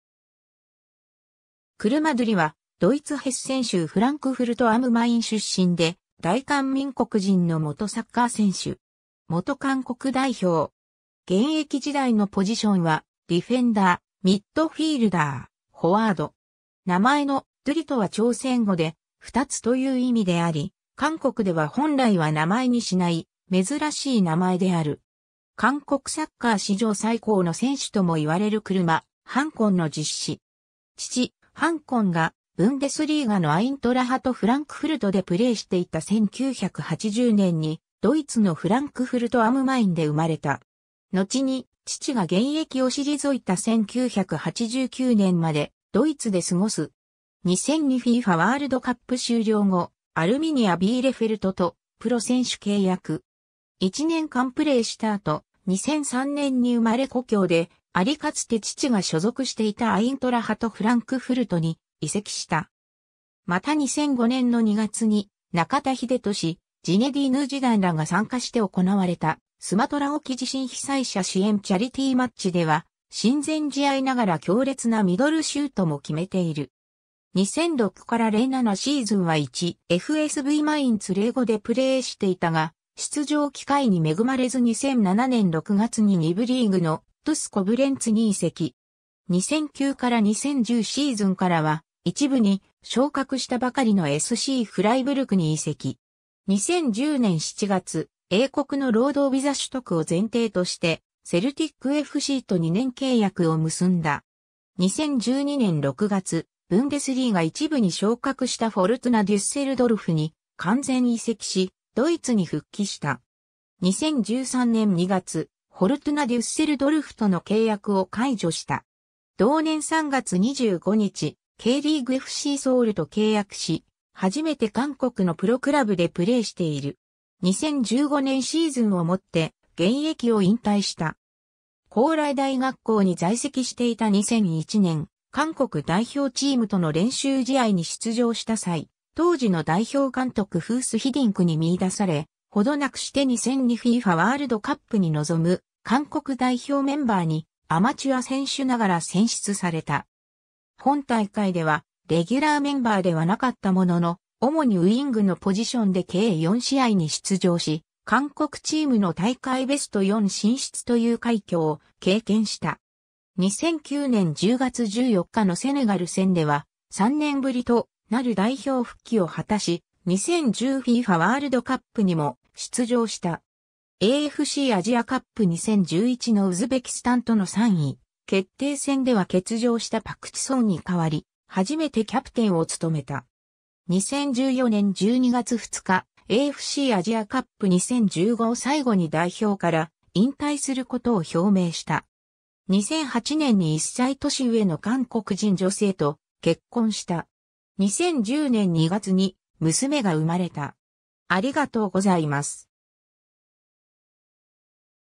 車ドゥリ
アルミニア・ビーレフェルトの選手
車 ドゥリ （チャ・ドゥリ, 차두리, 1980年7月25日 - ） は、ドイツヘッセン州フランクフルト・アム・マイン出身で大韓民国人の元サッカー選手。
元韓国代表。
現役時代のポジションはディフェンダー、ミッドフィールダー、フォワード。
 名前の「ドゥリ」とは朝鮮語で「ふたつ」という意味であり（姉はハナ:1つ、弟はセチ:3つ）、韓国では本来は名前にしない珍しい名前である。
 韓国サッカー史上最高の選手とも言われる車範根（チャ・ボングン）の実子。
父・範根がブンデスリーガのアイントラハト・フランクフルトでプレーしていた1980年にドイツのフランクフルト・アム・マインで生まれた。
のちに父が現役を退いた1989年までドイツで過ごす。
 2002 FIFAワールドカップ終了後、アルミニア・ビーレフェルトとプロ選手契約。
1年間プレーした後、2003年に生まれ故郷でありかつて父が所属していたアイントラハト・フランクフルトに移籍した。
また2005年の2月に中田英寿、ジネディーヌ・ジダンらが参加して行われた『スマトラ沖地震被災者支援チャリティーマッチ』では、親善試合ながら強烈なミドルシュートも決めている。
2006-07シーズンは1.FSVマインツ05でプレーしていたが出場機会に恵まれず2007年6月に2部リーグのTuSコブレンツに移籍。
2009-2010シーズンからは、1部に昇格したばかりのSCフライブルクに移籍。
 2010年7月、英国の労働ビザ取得を前提としてセルティックFCと2年契約（1年の延長オプション付き）を結んだ。
 2012年6月、ブンデスリーガ1部に昇格したフォルトゥナ・デュッセルドルフに完全移籍しドイツに復帰した。
 2013年2月、フォルトゥナ・デュッセルドルフとの契約を解除した。
同年3月25日、Kリーグ・FCソウルと契約し、初めて韓国のプロクラブでプレーしている。
 2015年シーズンをもって現役を引退した。
 高麗大学校に在籍していた2001年、韓国代表チームとの練習試合に出場した際、当時の代表監督フース・ヒディンクに見出され、程なくして2002 FIFAワールドカップに臨む韓国代表メンバーにアマチュア選手ながら選出された。
本大会ではレギュラーメンバーではなかったものの、主にウイングのポジションで計4試合に出場し、韓国チームの大会ベスト4進出という快挙を経験した。
 2009年10月14日のセネガル戦では、3年ぶりとなる代表復帰を果たし、2010 FIFAワールドカップにも出場した。
AFCアジアカップ2011のウズベキスタンとの3位決定戦では欠場したパク・チソンに代わり、初めてキャプテンを務めた。
 2014年12月2日、AFCアジアカップ2015を最後に代表から引退することを表明した。
 2008年に1歳年上の韓国人女性と結婚した。
2010年2月に娘が生まれた。